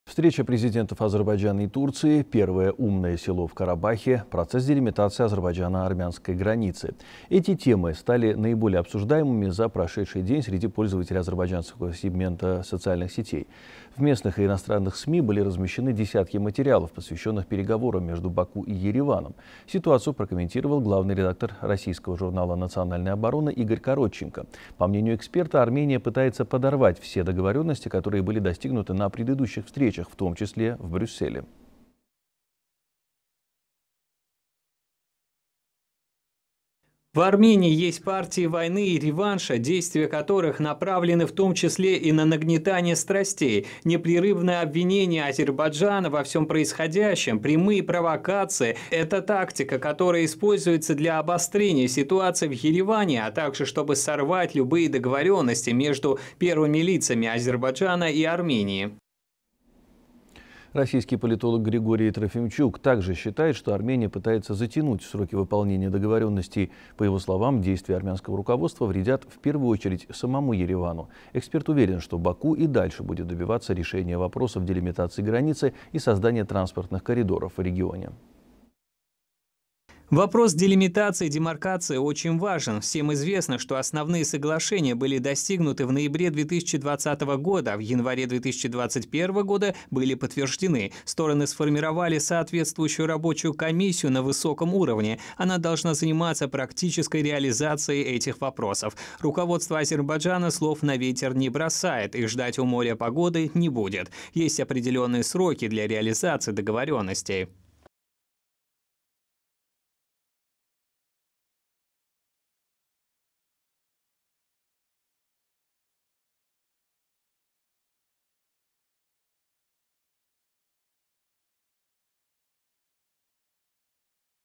Делимитация азербайджано-армянской границы
Встреча президентов Азербайджана и Турции, первое «умное село» в Карабахе, процесс делимитации азербайджано-армянской границы. В местных и иностранных СМИ были размещены десятки материалов, посвященных переговорам между Баку и Ереваном.

Ситуацию прокомментировал главный редактор российского журнала «Национальная оборона» Игорь Коротченко. По его мнению, Армения пытается подорвать все договоренности, которые были достигнуты на предыдущих встречах, в том числе в Брюсселе, но "есть определённые сроки для реализации договоренностей", - подчеркнул он.